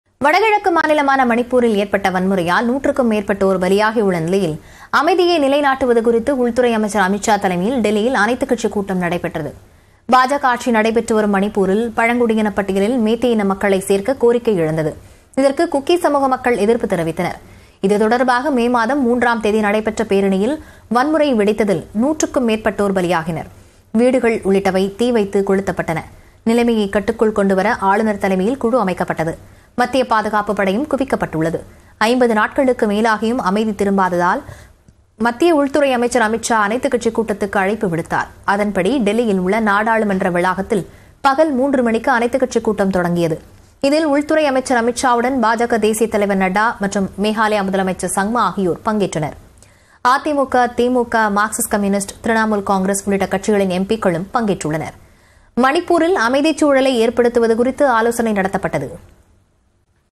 கட்டுக்குள் வருமா மணிப்பூர் கலவரம்? - அனைத்துக்கட்சி கூட்ட ஆலோசனை | Tamil News
கட்டுக்குள் வருமா மணிப்பூர் கலவரம்? - அனைத்துக்கட்சி கூட்ட ஆலோசனை

News18TamilNadu | Manipur | All Party Meeting | Manipur Violence | Riot | Tamil News